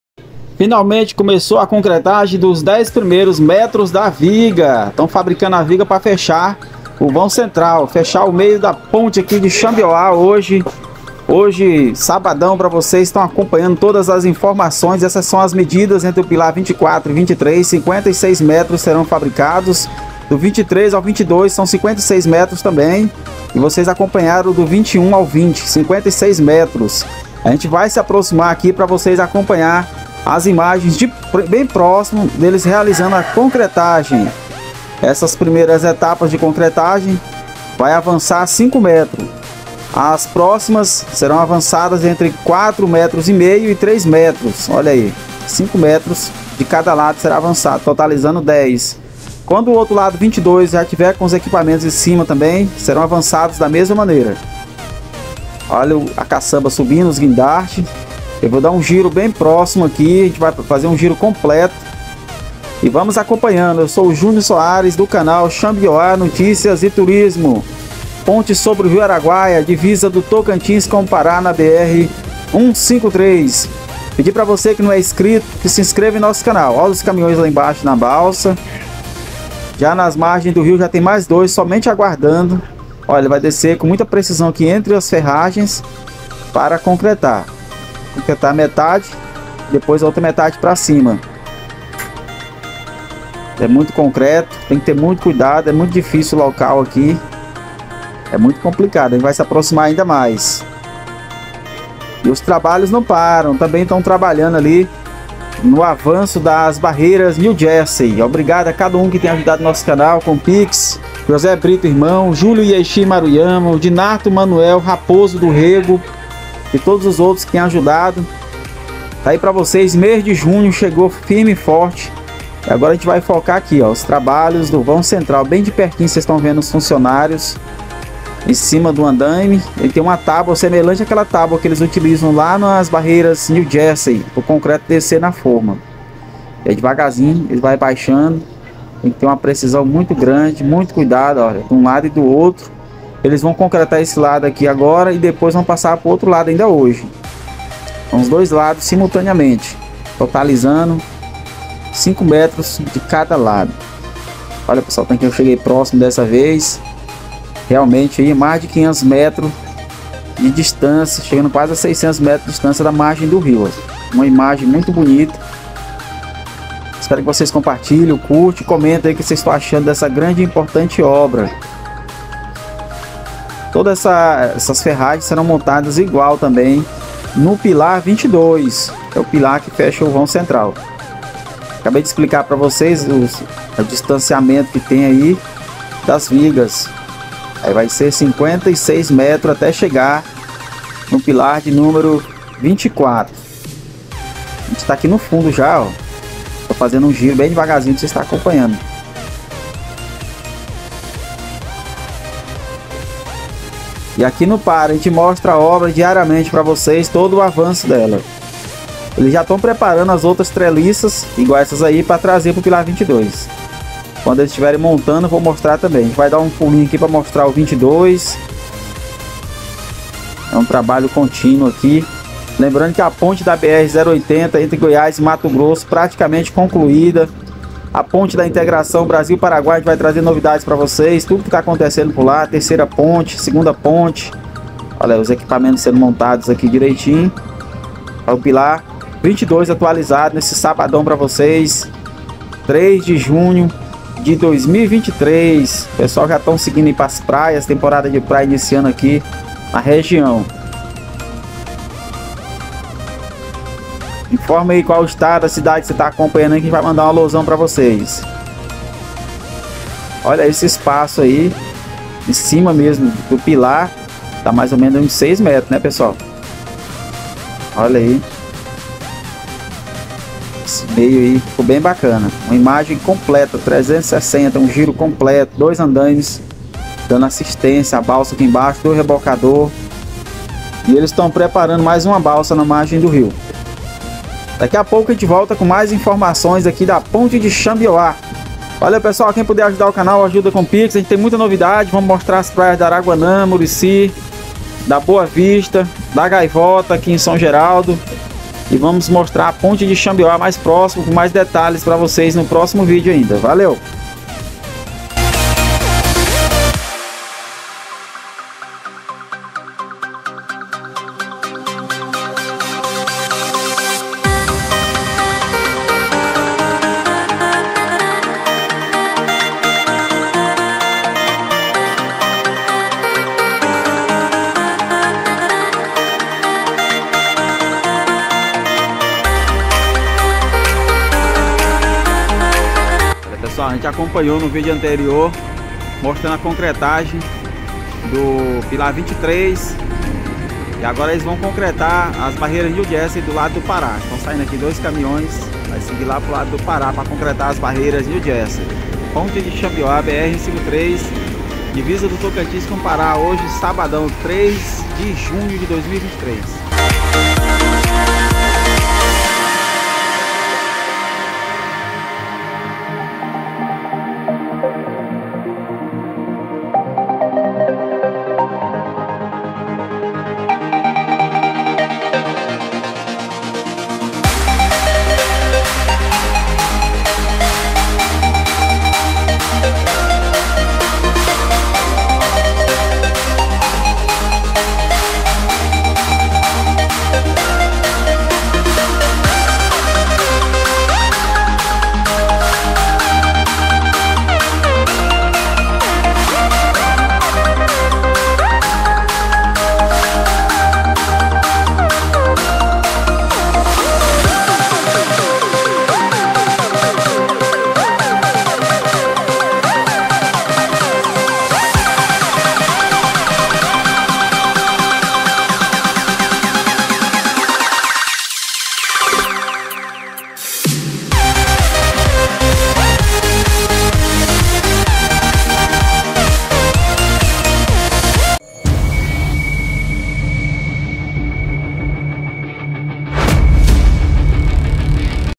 🌍 CONCRETAGEM DOS 10 PRIMEIROS METROS DO MEIO DA PONTE /AGORA SOBRE O RIO ARAGUAIA "VÃO CENTRAL"
👉FAÇA A DIVULGAÇÃO "PROPAGANDA" PARCERIA DE SUA EMPRESA EM NOSSO CANAL ATRAVÉS DESTE CONTATO ZAP (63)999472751
JÚNIOR SOARES

celular 👉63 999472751 Júnior Soares